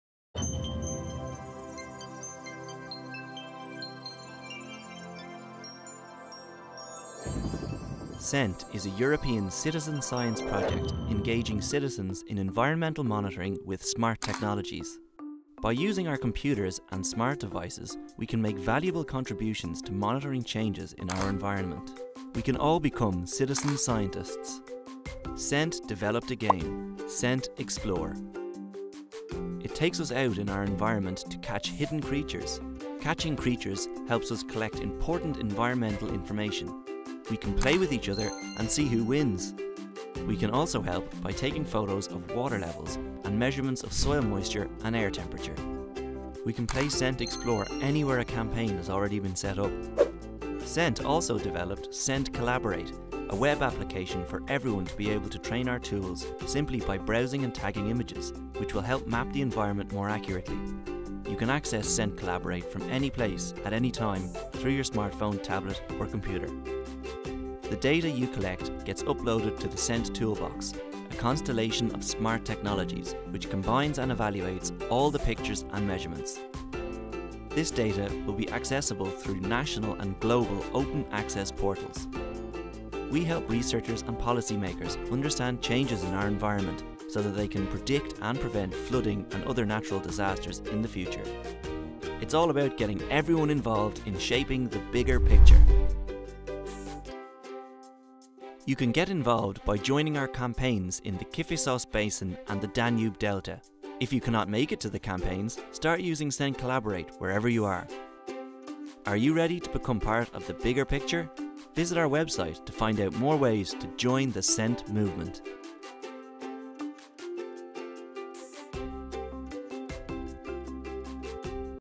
Scent Animation
Scent is a European Union funded research project creating new innovative tools and systems to enable citizens to participate in environmental monitoring.

Scent has developed fun new gaming apps, including Scent Explore and Scent Measure, to help citizens monitor changes in their local environment and to help scientists understand how these changes affect flooding.

Scent is a European Union research and innovation project, which is funded under the EU's Horizon 2020 research and innovation programme under grant agreement no. 688930.